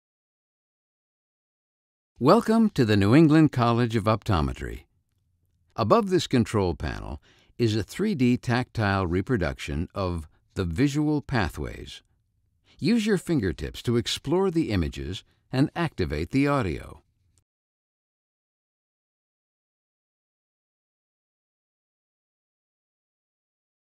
New England College of Optometry goes tactile with image of the brain and visual pathways
The New England College of Optometry (NECO) Center for Eye Care has a new interactive experience featuring a tactile image of the visual system on display at 930 Commonwealth Ave. A tactile image allows touch and sound to stimulate a sensory experience for the brain to create a visual image that allows a person to ‘see’. The visual system is complex, but this learning tool helps individuals understand the inner-workings of the brain that create a visual understanding of the world.